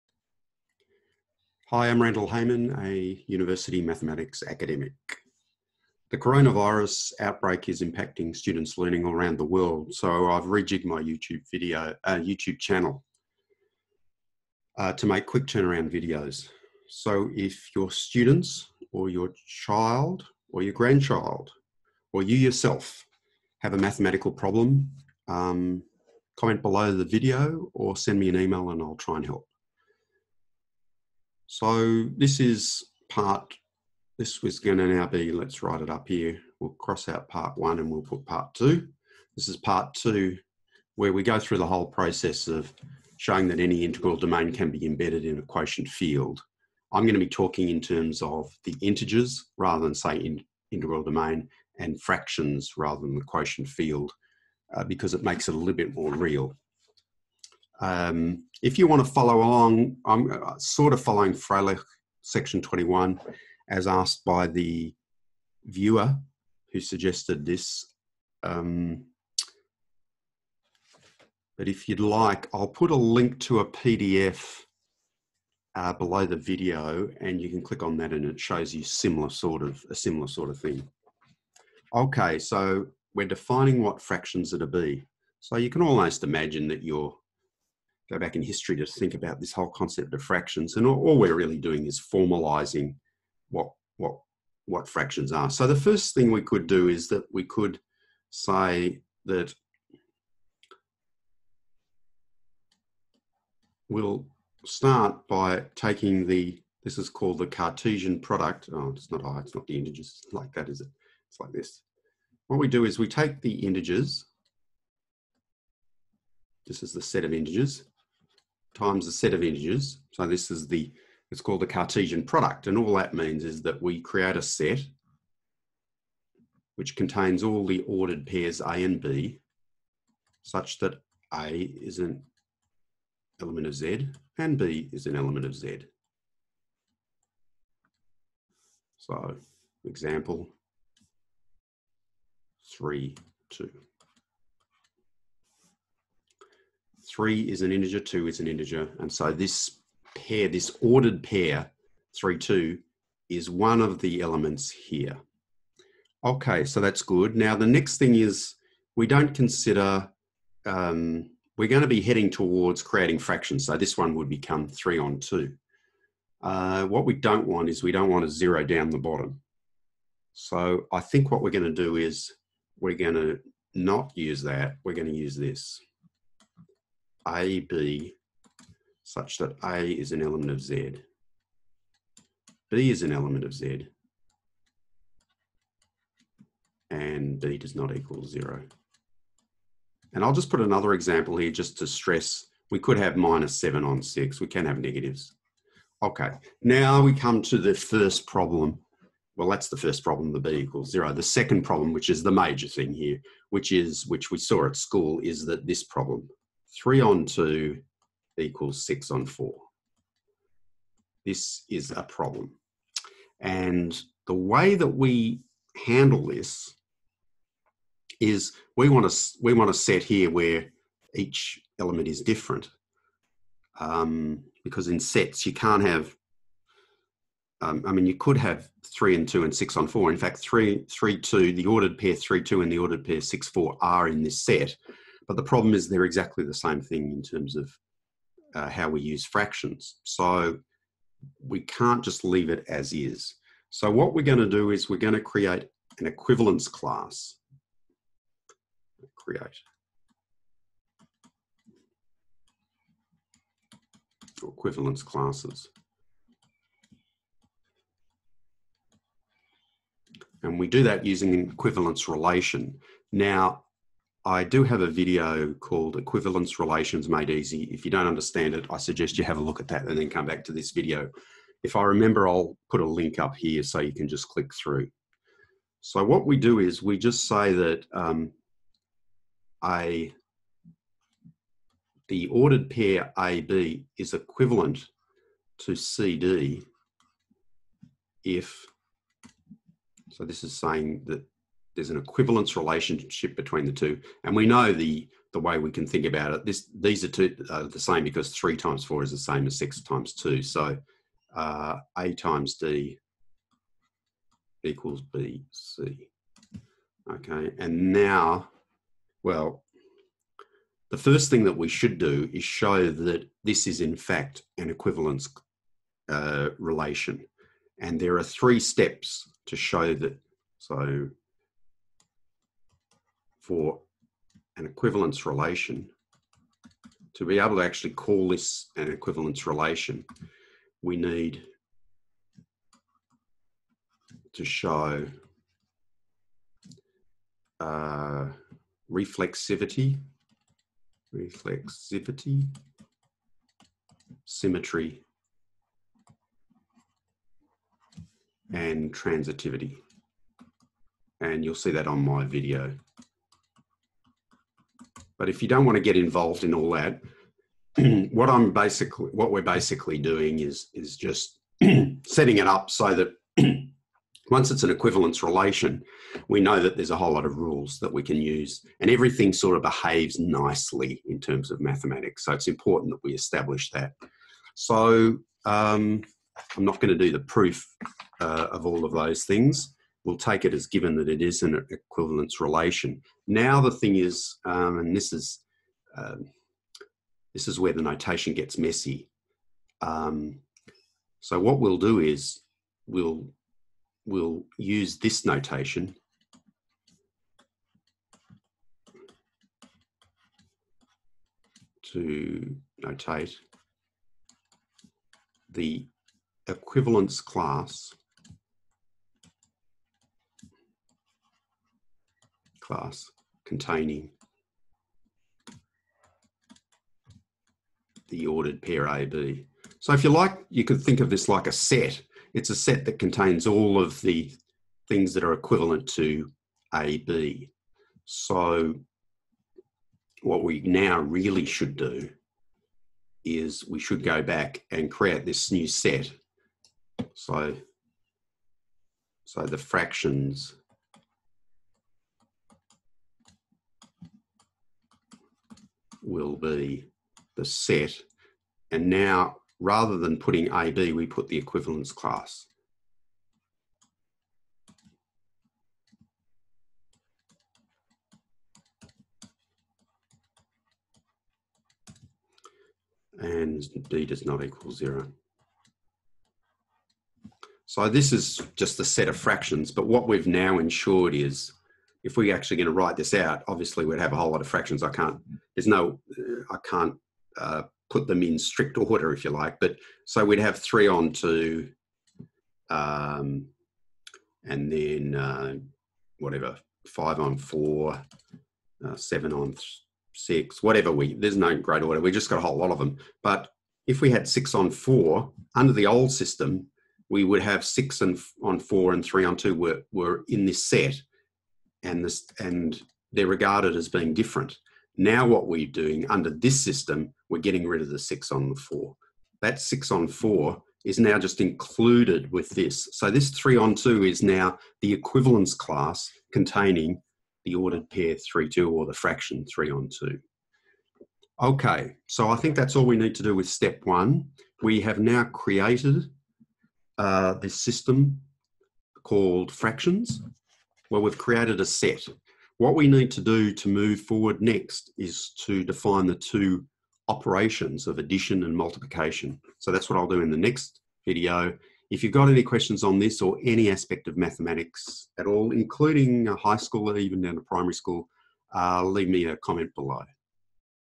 The quotient field of an integral domain - part 2
I explain how to construct the set of fractions from the integers recognising that 2 fractions (e.g. 3/2 and 6/4) can have equal value.